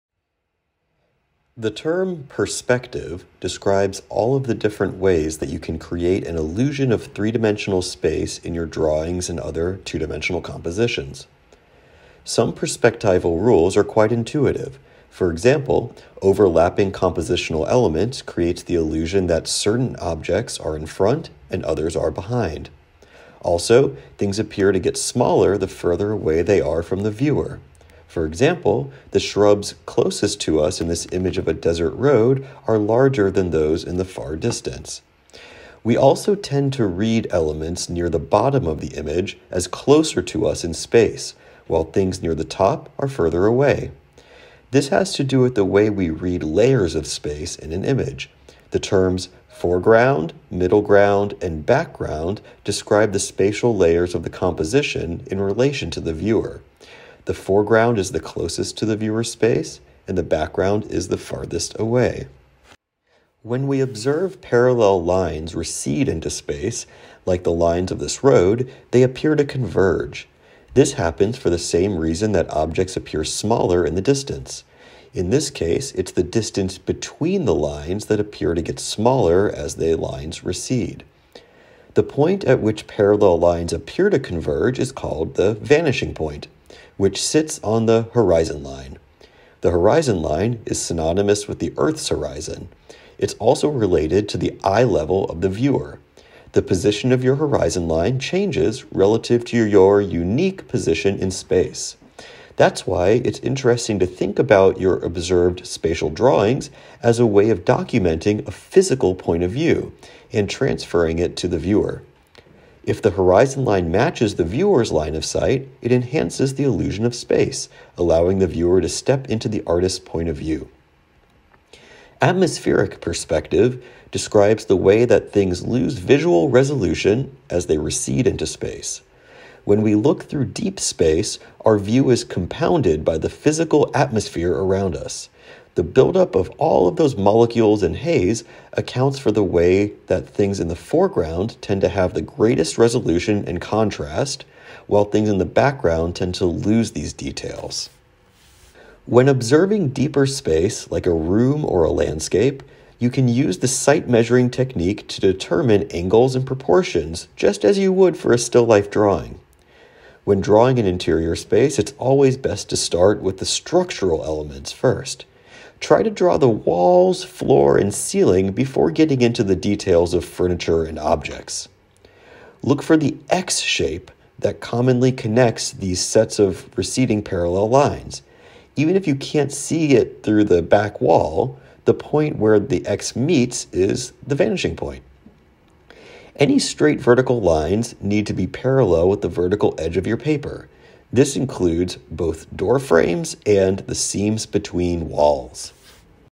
Drawing Deep Space: Perspective
Use methods of perspective to draw 3-dimensional spaces from observation. Learn about both linear and atmospheric perspective drawing methods.